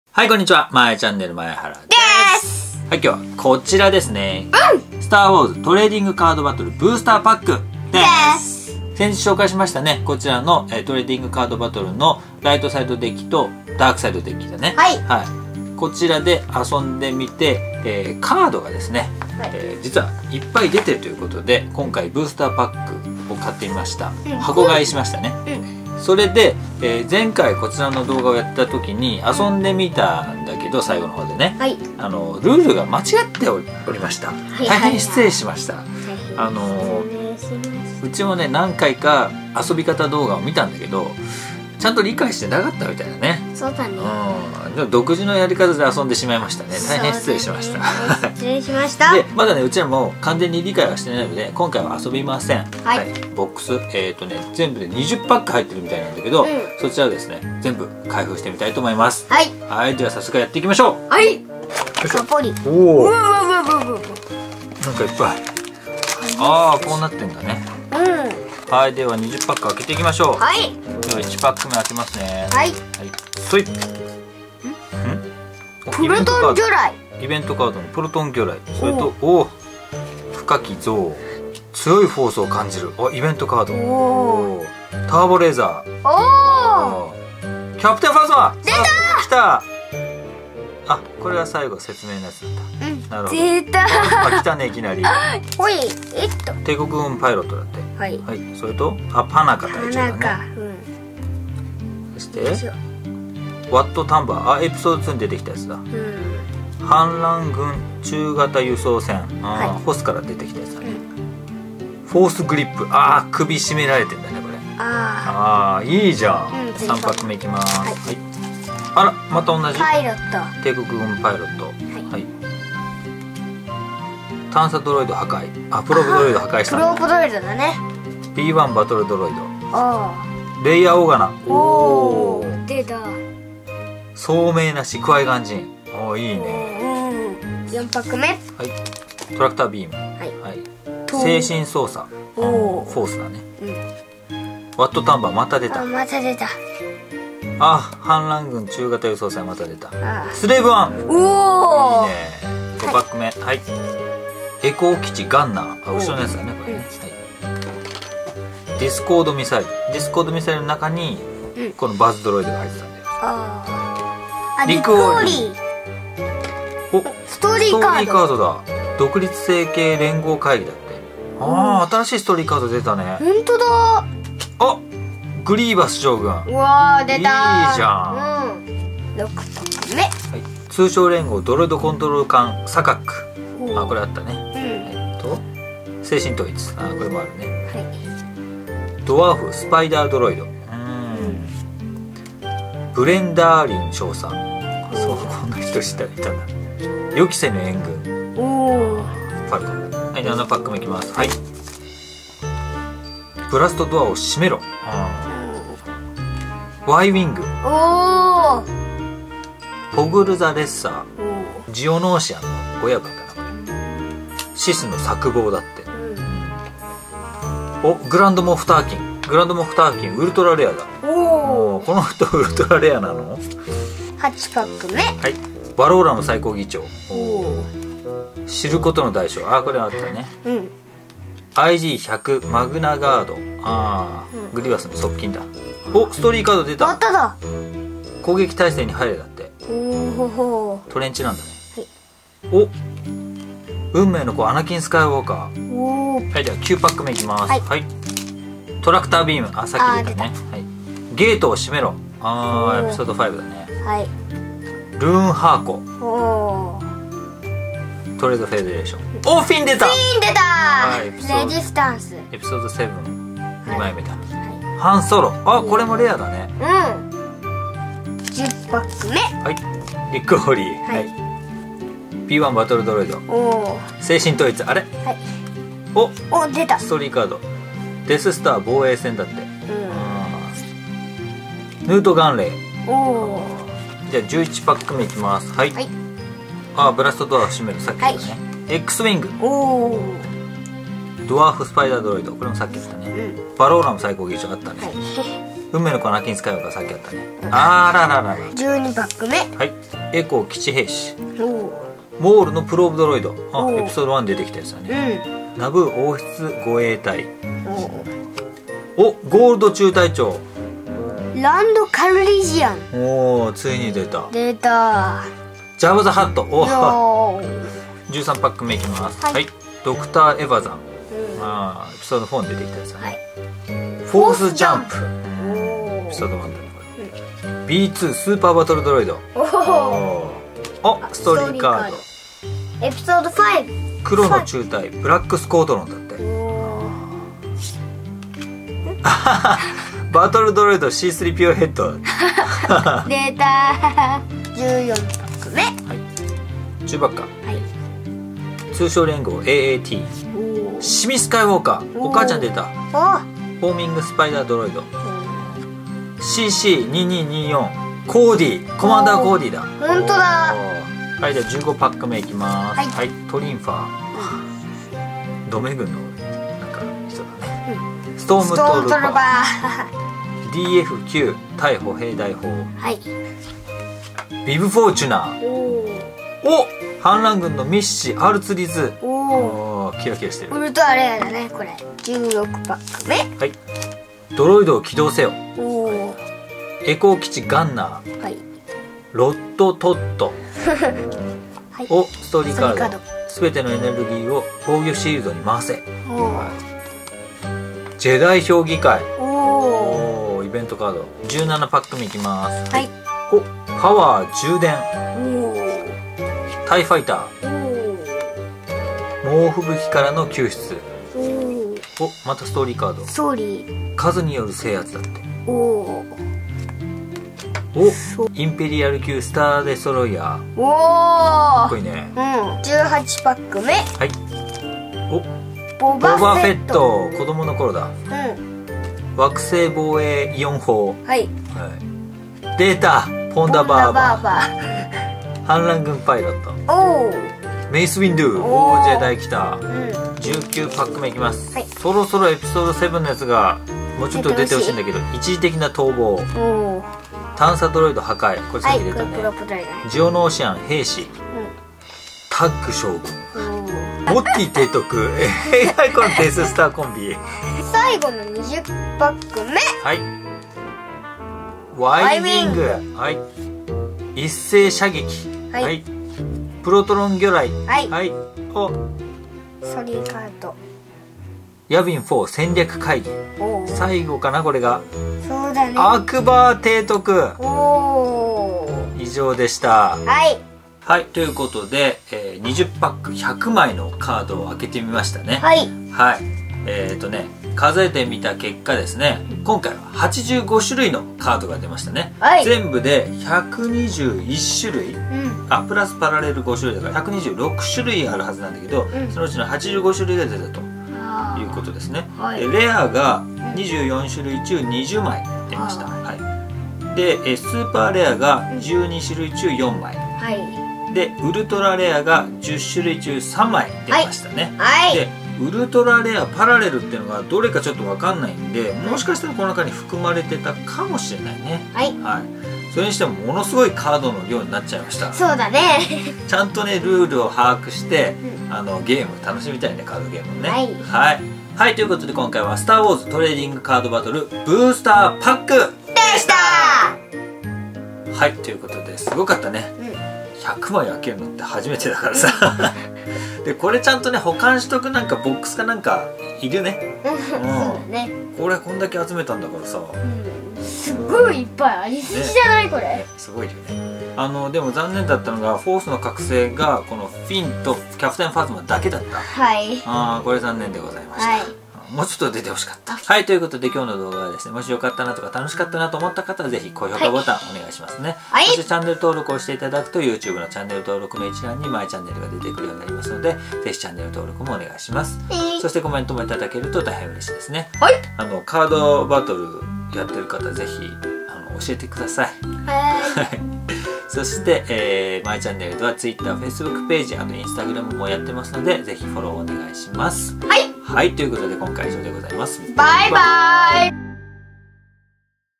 フォースの覚醒特別収録☆ 20パック100枚一気に開封！ スター・ウォーズ トレーディングカードバトル BOX開封 STAR WARS Trading Card Battle
スター・ウォーズ トレーディングカードバトルのブースターパックをBOX購入しました！
今回は20パック100枚を一気に開封しております^^

前回構築済みスターターの「ライトサイドデッキ」と「ダークサイドデッキ」を紹介して、最後の方にカードゲームで遊んだのですが、どうやらかなり間違えていたようです。
大変失礼いたしました。orz

未だにきちんとしたルールが理解できていないので今回は開封のみでございます！

▽STAR WARS Trading Card Battle(仮) ブースターパック 【SW-01】(BOX)（Amazon）

▽使用音楽（YouTube オーディオ ライブラリ）

▽撮影機材　SONY α NEX-5R（Amazon）